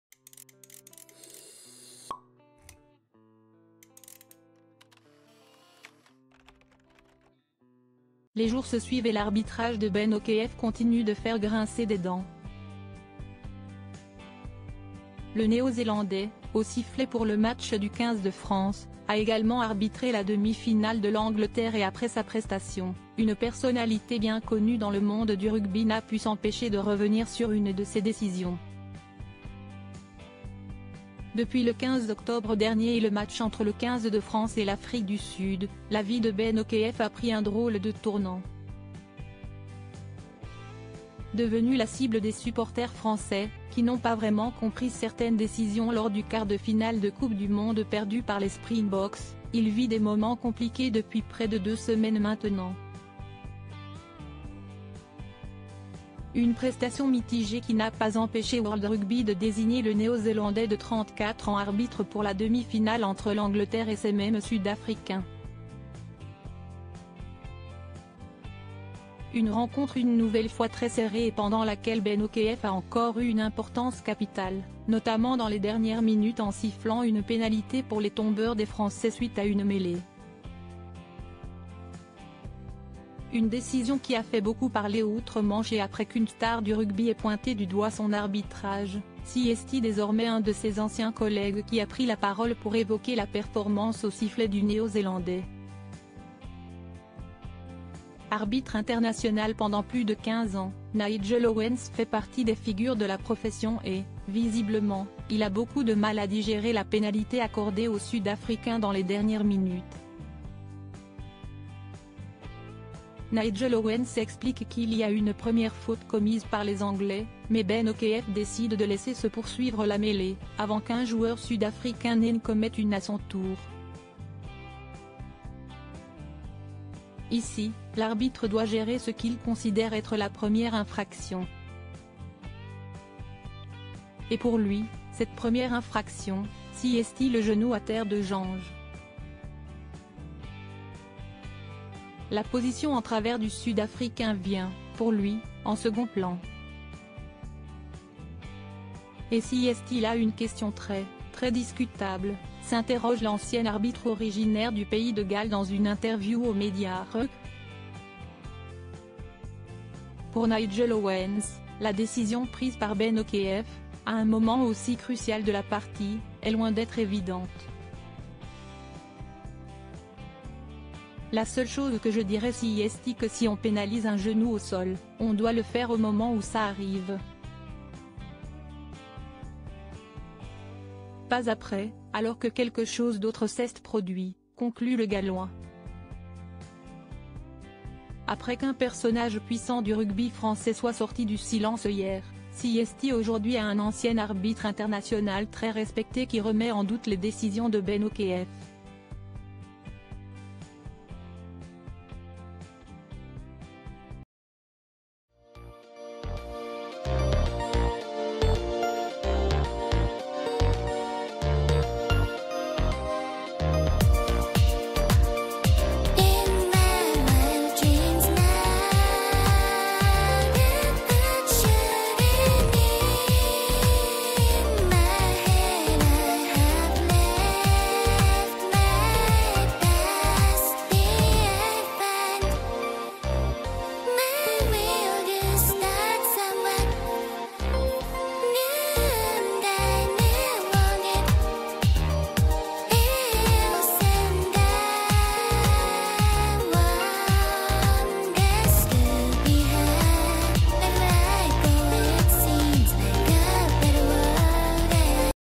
Très très discutable Ben O'Keeffe, l'arbitre de France Afrique du Sud, encore mis en doute par
"Très très discutable" : Ben O'Keeffe, l'arbitre de France-Afrique du Sud, (encore) mis en doute par une sommité du rugby